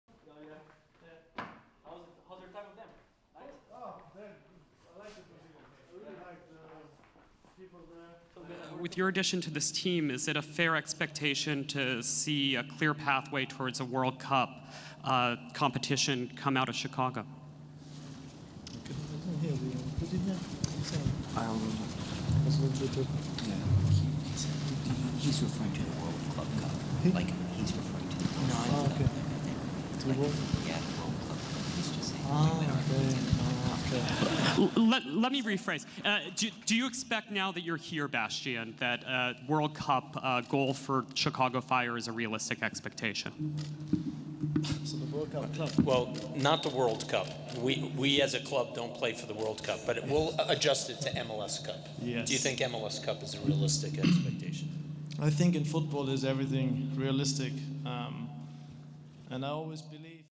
Швайнштайгер. "Труднощі перекладу"
Бастіан Швайнштайгер втрапив в історію на рівному місці. "Місто вітрів" славиться своїми спортивними досягненнями у хокеї, бейсболі, американському футболі, баскетболі. А от МЛС, як виявилося, слабке місце тамтешніх репортерів.